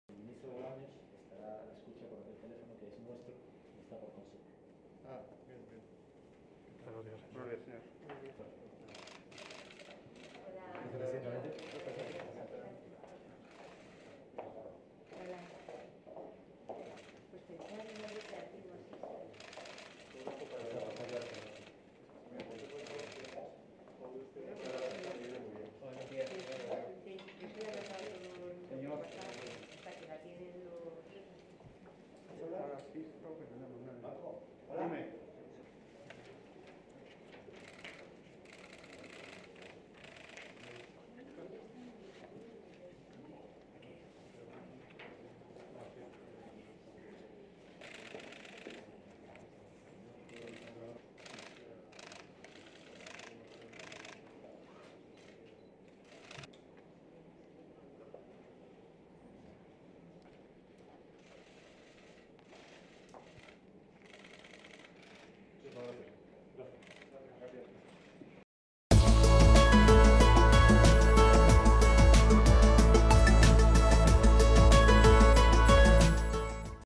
El rey Felipe VI preside la reunión del Consejo de Seguridad Nacional por la crisis de Ucrania
Felipe VI ha presidido este jueves la reunión del Consejo de Seguridad Nacional, celebrada en el Palacio de la Zarzuela de forma extraordinaria tras el ataque de Rusia a Ucrania.